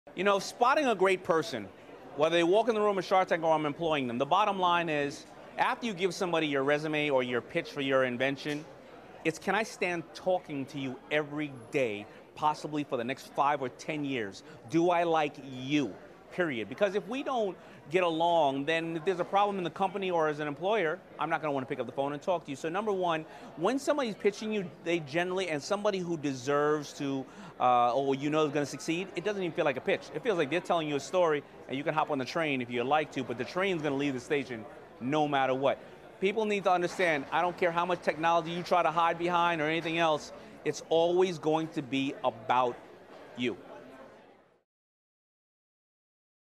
The 1 Thing that Will Get You Hired by Daymond John
Shark Tank's Daymond John knows what makes a good employee.